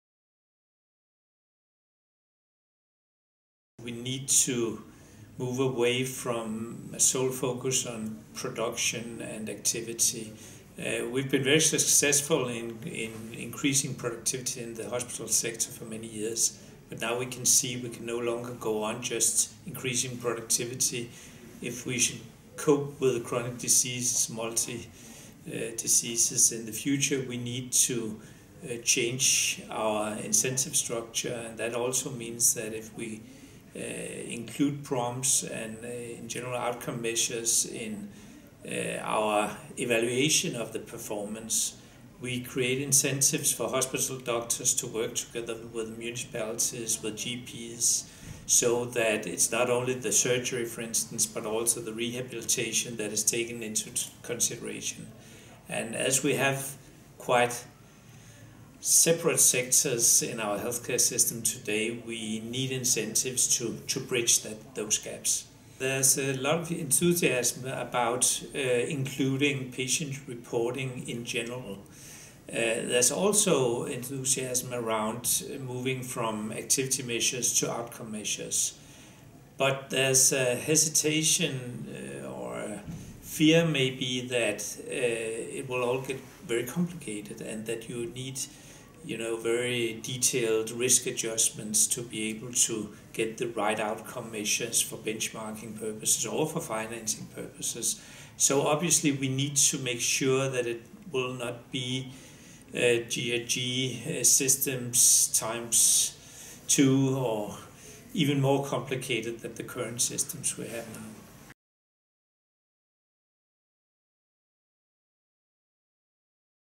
Pilot projects in Denmark use patient outcomes to help reform the healthcare system
Danish regions are experimenting with new approaches to using patient reported outcome measures and financing healthcare services, Adam Wolf, CEO of Danish Regions tells Science|Business in an interview.